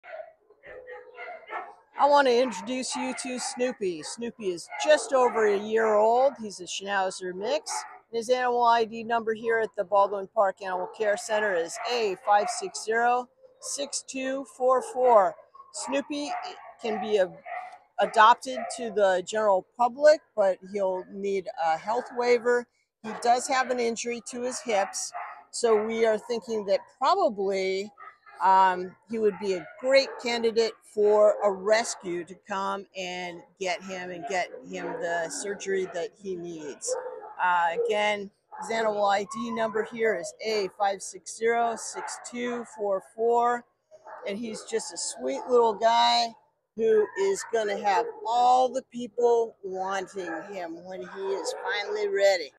NEW! A5606244 Snoopy | Miniature Schnauzer/Terrier Mix
A5606244 Snoopy is a scruffy cutie with a fractured femoral head, but he’s not letting that injury get him down! Waggy and happy, he’s a darling teen puppy just over one year. He needs to be seen by an outside vet for a treatment plan and a health waiver is required.
- frisky and happy to explore on leash, pulls a little but easy to handle
- just 21 pounds, a great size for any type of home
- wants to play! Fetch or just running around make him happy
- shows signs of housebreaking
- curious about other dogs, barks a happy hello!
- needs all the puppy basic training
- black and tan, adorable face and ears
- not yet neutered, trust waiver may be required if he hasn’t been neutered by his adoption date
- brought as a stray to the Baldwin Park Animal Care Center on January 31